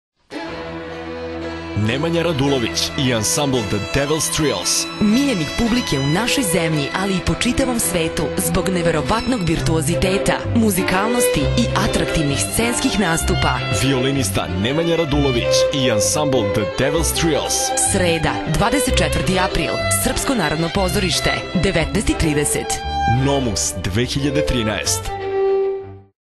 NOMUS 2013 - Nemanja Radulovic & The Devil’s Trills Concert advertisement
Najava koncerta Nemanje Radulovića (violina) i ansambla "The Devil’s Trills" (Srbija/Francuska)

Nemanja Radulović, violina
Guillaume Fontanarosa, violina
Frederic Dessus, violina
Bernard Causse, viola
Anne Biragnet, violončelo
Boban Stošić, kontrabas